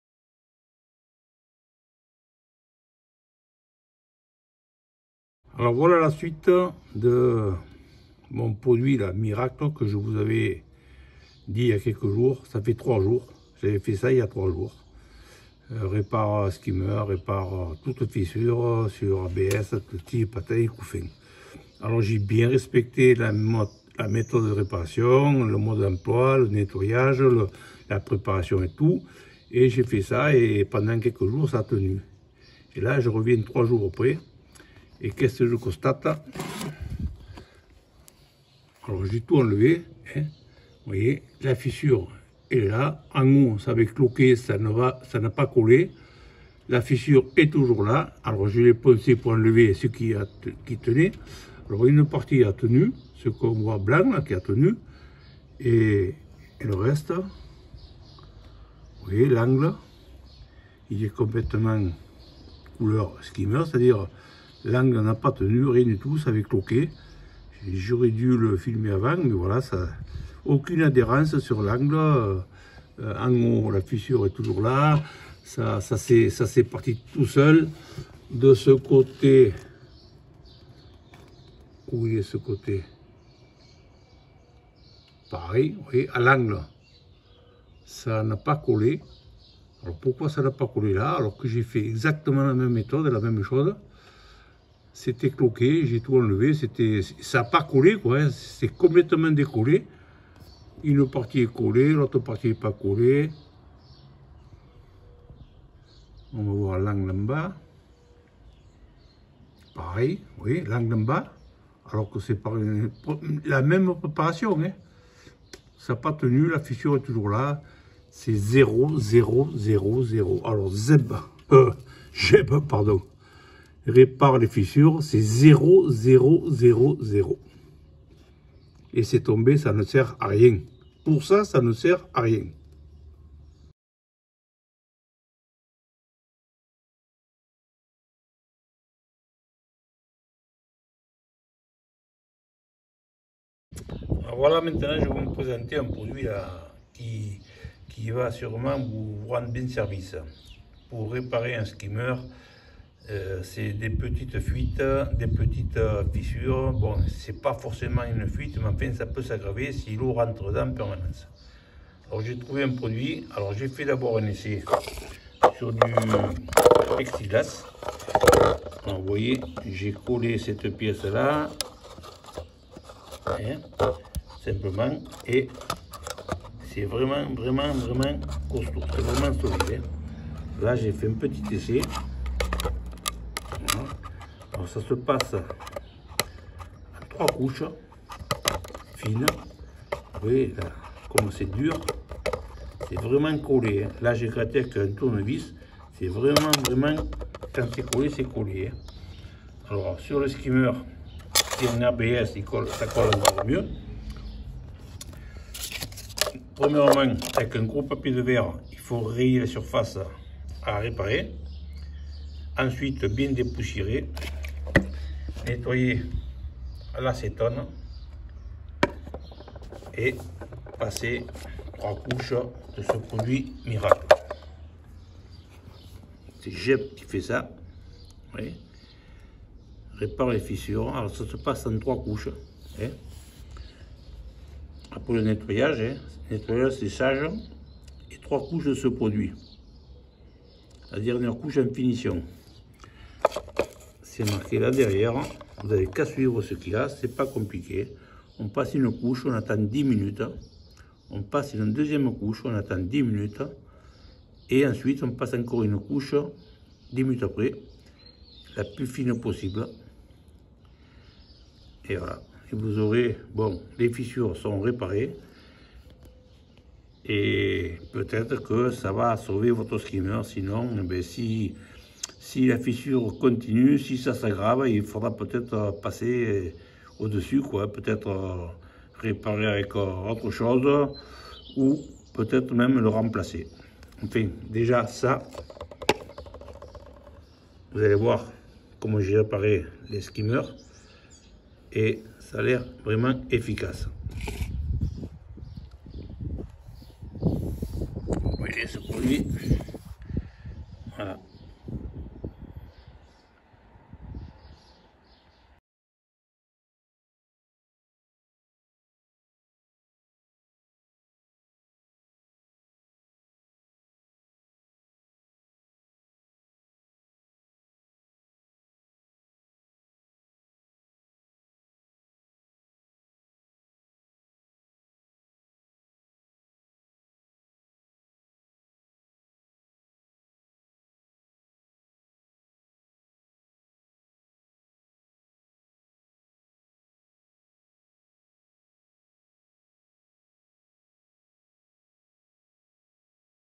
GEB RÉPARE FISSURES ZÉRO aucune efficacité
Le produit que je vous ai présenté GEB répare fissures en fait 3 jours après c’est la grosse arnaque, catastrophe, aucune efficacité, voir la vidéo du résultat.